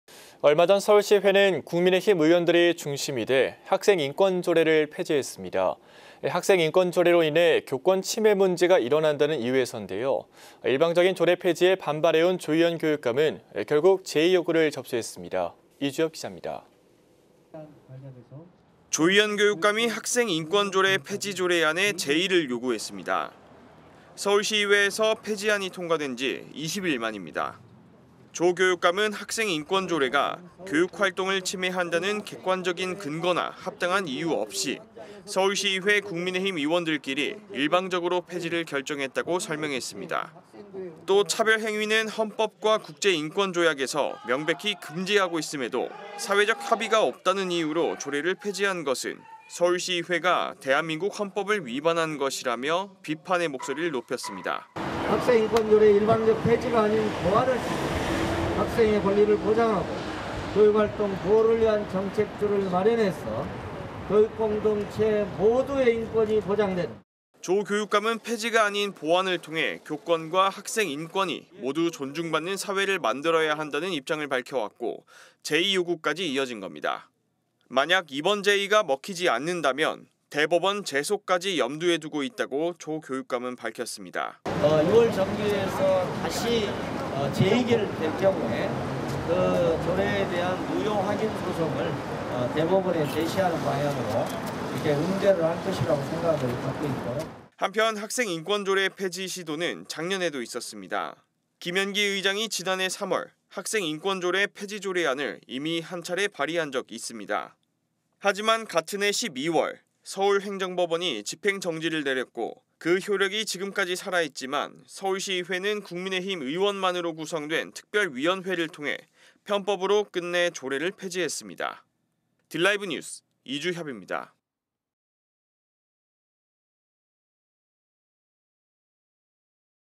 조희연, 학생인권조례 폐지 재의요구…"폐지 아닌 보완" / 딜라이브 뉴스
진행 : 박세종 아나운서

【 앵커멘트 】
얼마 전 서울시의회는
국민의힘 의원들이 중심이 돼
학생인권조례를 폐지했습니다.
학생인권조례로 인해
교권 침해 문제가
일어난다는 이유에서인데요.
일방적인 조례 폐지에
반발해온 조희연 교육감이
결국 재의 요구를 접수했습니다.
이주협 기자의 보도입니다.

【 VCR 】
조희연 교육감이
학생인권조례 폐지 조례안의
재의를 요구했습니다.

서울시의회에서 폐지안이 통과된 지
20일 만입니다.

조 교육감은
학생인권조례가 교육활동을 침해한다는
객관적인 근거나 합당한 이유 없이,
서울시의회 국민의힘 의원들끼리
일방적으로 폐지를 결정했다고 설명했습니다.

또 차별행위는 헌법과 국제인권조약에서
명백히 금지하고 있음에도
사회적 합의가 없다는 이유로
조례를 폐지한 것은
서울시의회가 대한민국 헌법을 위반한 것이라며
비판의 목소리를 높였습니다.

【 인터뷰 】
( 조희연 서울시교육감 )
"학생인권조례의 일방적 폐지가 아닌 보완을 통해
학생의 권리를 보장하고, 교육활동 보호를 위한 정책들을 마련해서
교육공동체 모두의 인권이 보장되는 선진인권도시의 길을…."

【 VCR 】
조 교육감은 폐지가 아닌 보완을 통해
교권과 학생인권이 모두 존중받는
사회를 만들어야 한다는 입장을 밝혀왔고
재의 요구까지 이어진 겁니다.

만약 이번 재의가 먹히지 않는다면
대법원 제소까지 염두에 두고 있다고
조 교육감은 밝혔습니다.

【 인터뷰 】
"6월 정례회에서 다시 재의결이 될 경우에
그 조례에 대한 무효 확인 소송을 대법원에 제소하는 방향으로
응대할 것이라 생각하고 있고요…."

【 VCR 】
한편, 학생인권조례 폐지 시도는
작년에도 있었습니다.

김현기 의장이 지난해 3월
학생인권조례 폐지조례안을
이미 한차례 발의한 적 있습니다.

하지만 같은 해 12월
서울행정법원이 집행정지를 내렸고
그 효력이 지금까지 살아있지만,
서울시의회는
국민의힘 의원만으로 구성된
특별위원회를 통해 편법으로
끝내 조례를 폐지했습니다.

딜라이브 뉴스 이주협입니다.